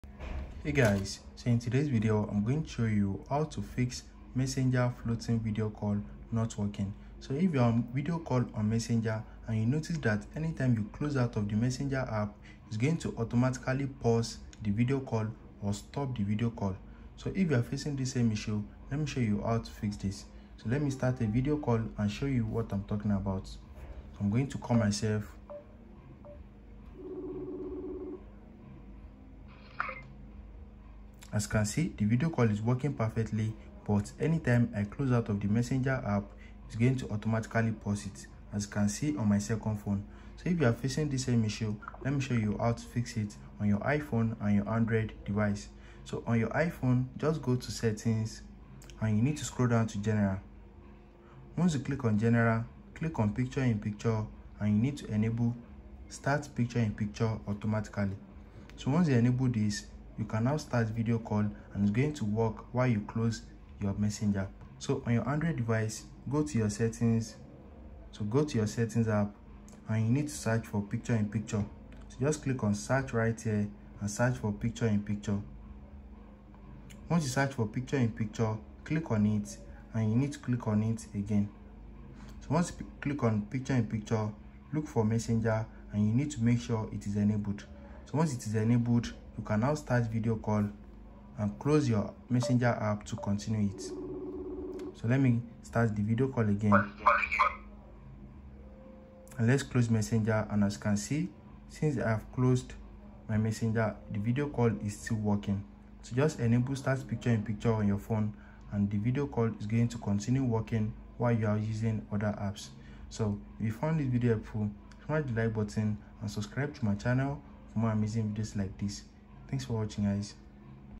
How to Fix Messenger Floating Video Call Not Working (iPhone & Android)
Encountering issues with Messenger's floating video call feature on your iPhone or Android device? Don't worry, we've got you covered! Learn how to troubleshoot and fix it with our step-by-step guide. Watch now to ensure uninterrupted multitasking during your Messenger calls!